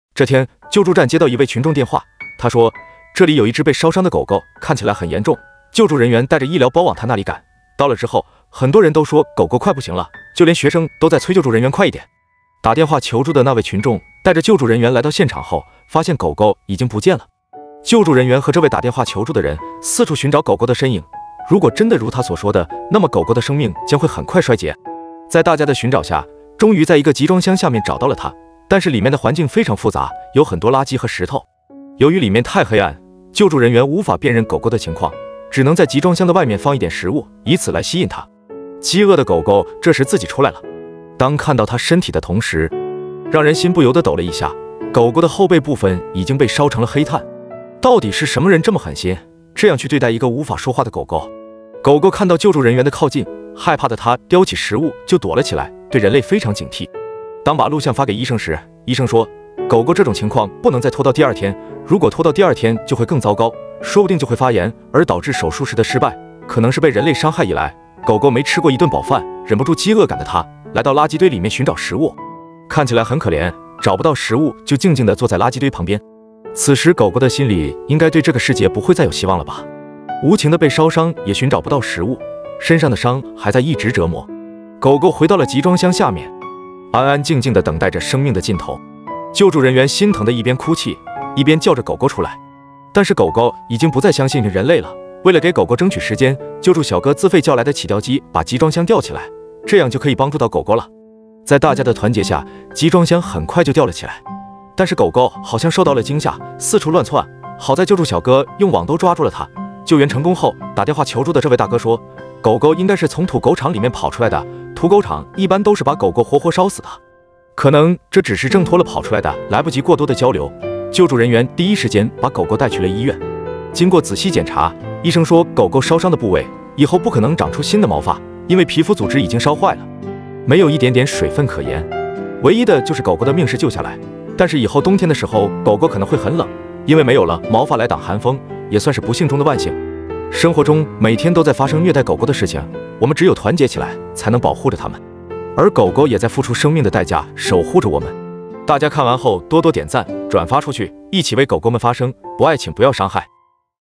屠宰場逃出來的狗狗還被人惡意燒傷，彷彿流下了傷痛的淚水，是什麼人可以做出這麼狠的事？ 【收養 救助 流浪狗 寵物 溫暖 暖心 善良 狗狗 正能量 動物 毛孩子 】
屠宰場逃出來的狗狗還被人惡意燒傷，彷彿流下了傷痛的淚水，是什麼人可以做出這麼狠的事？